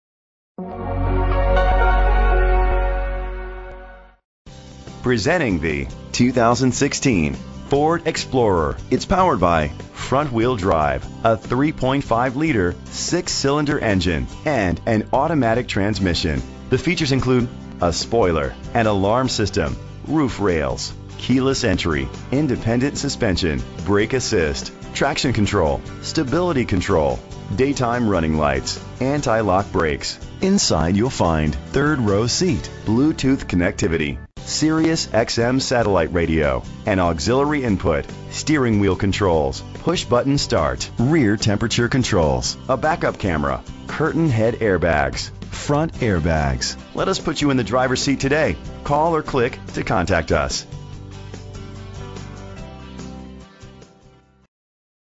2016 Ford Explorer NDG8882 - Orland Park IL
Year: 2016
Make: Ford
Model: Explorer
Trim: XLT
Engine: 3.5 liter 6 cylinder FWD
Transmission: Automatic With Overdrive
Color: Black
Mileage: 0
Address:
8100 West 159th StreetOrland Park, IL 60462
VIN:1FM5K7D81GGA01101